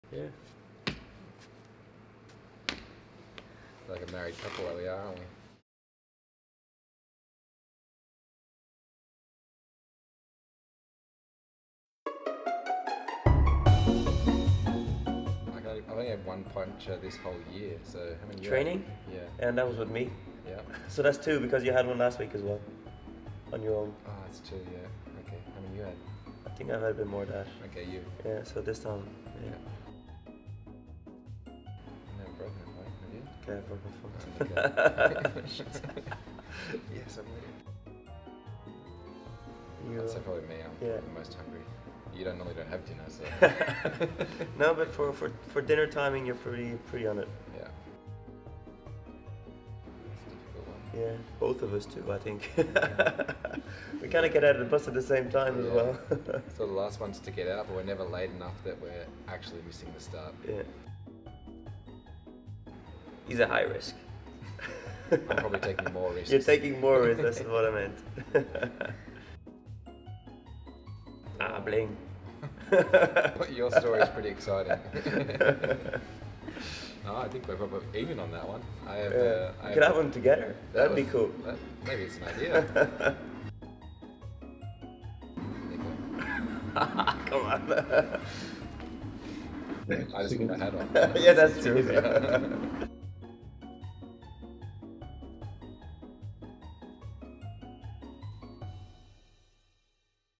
'Who's most likely to?' with Michael Matthews and Nicholas Roche | On The Sofa
Sit down and grab some popcorn for a hilarious OnTheSofa episode with Michael Matthews and Nicholas Roche as they play a game of "Who's most likely to?". Be prepared for lots of laughs from the Team Sunweb duo!